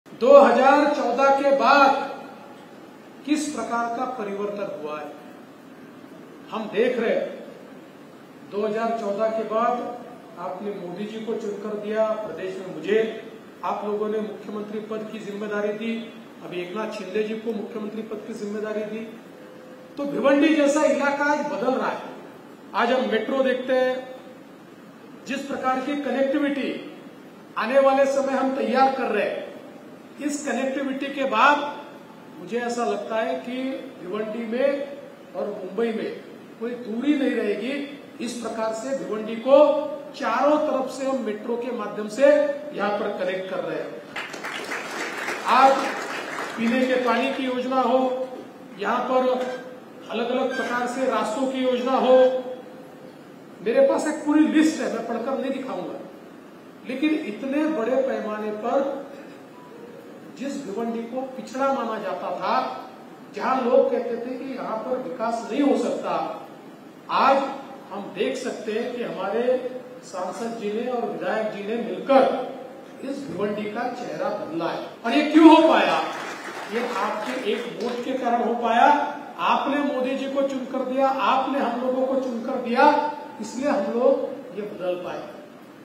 भिवंडी और मुंबई कनेक्टिविटी | DCM Devendra Fadnavis
(कपिल पाटील जी के प्रचार हेतु भाजपा जाहीर सभा | भिवंडी | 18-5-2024)